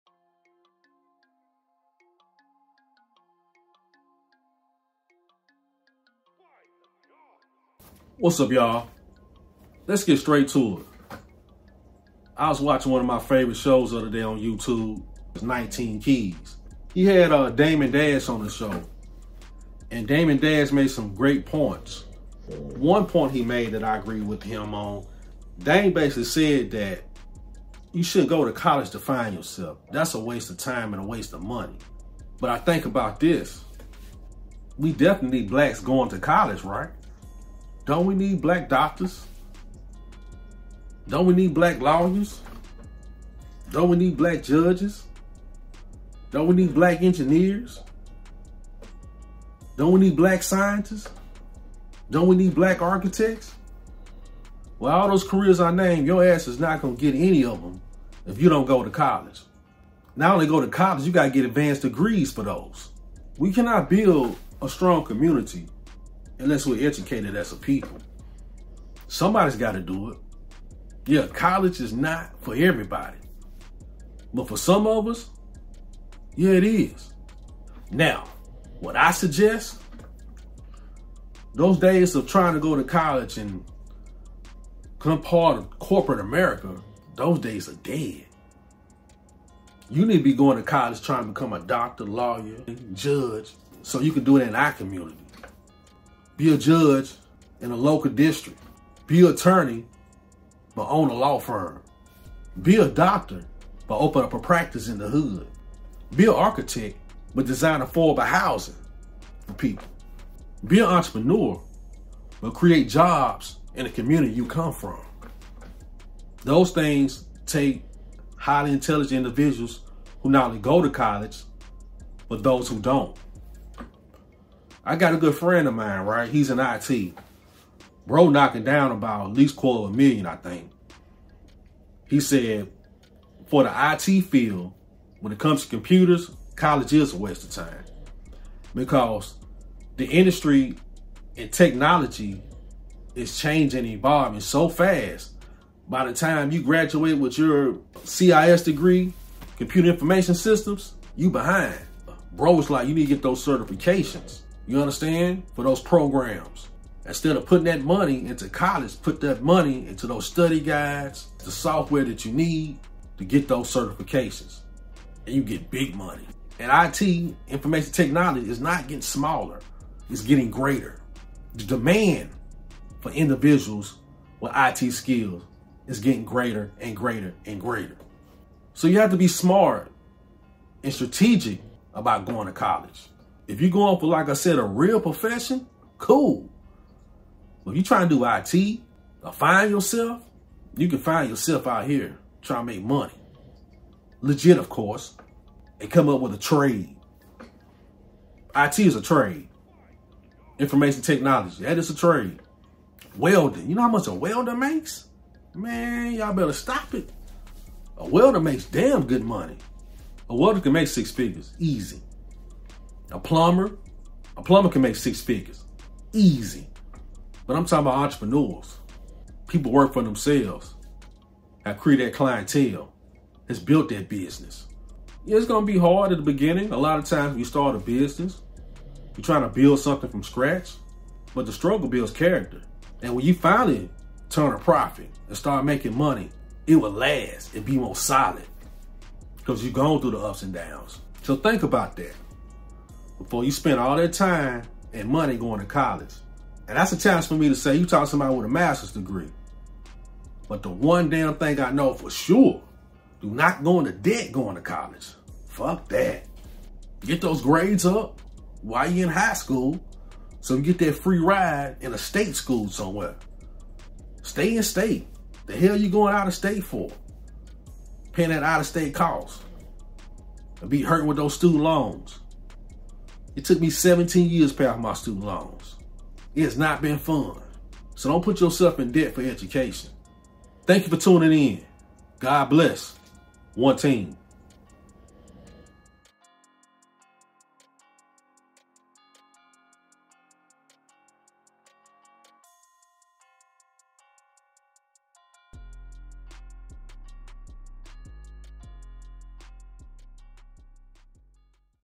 How College Has Become a WASTE of Time and MONEY??!!!
Edgar Alan Cole, author of Reporter from Hell, The Last Society, & The Black P.O.E., EXAMINES is college still important and necessary.